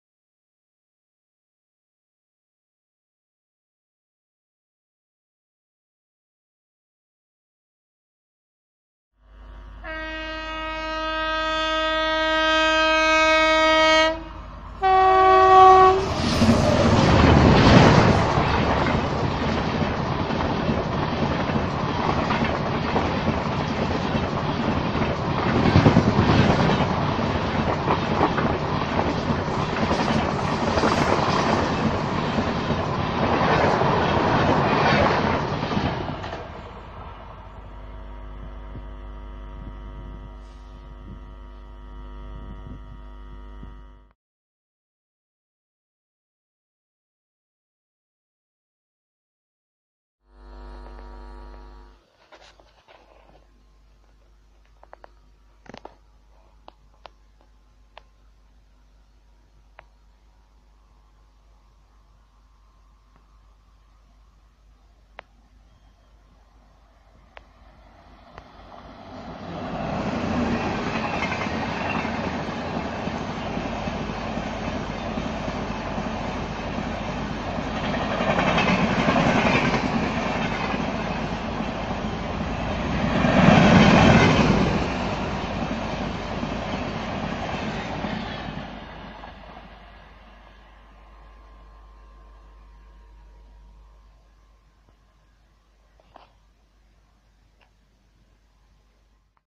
नए चमचमाते LHB Rake:1st & 2nd Newly LHB Fied!! 12450 Chandigarh Madgaon Goa Sampark Kranti Express
12450 Chandigarh Madgaon Goa Sampark Kranti Express First and Second Rake Running
Loco BRC &  GZB  Wap7